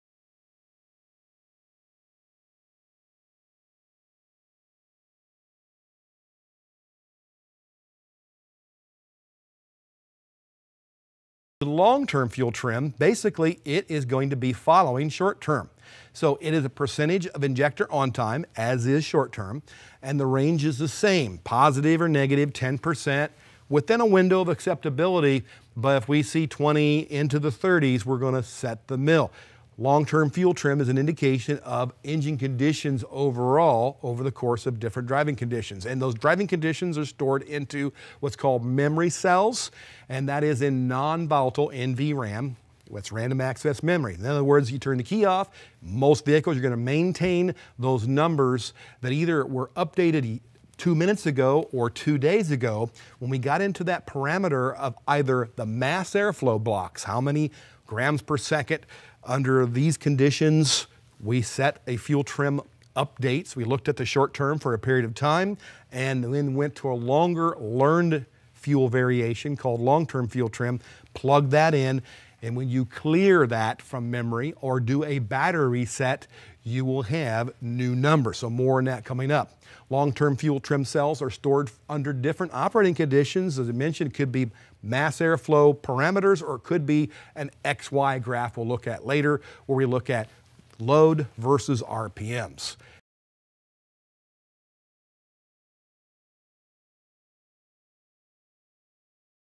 Long Term Trim Code 101 (Diagnosing to Win with Fuel Trim)
Need a primer on long term trim code? The Delphi Training Series breaks it down for you.